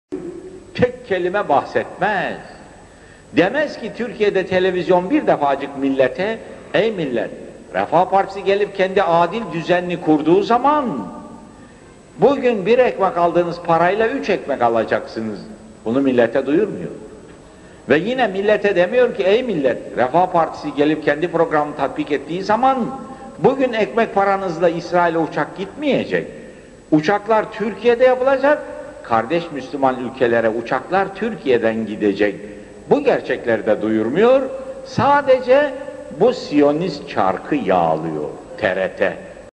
TRT siyonist çarkı yağlıyor!
Avrupa ve Islam-Avrupa ve diaspora adlı konferanstan  bir kesit
Necmettin Erbakan - Almanya (Wuppertal) - 1991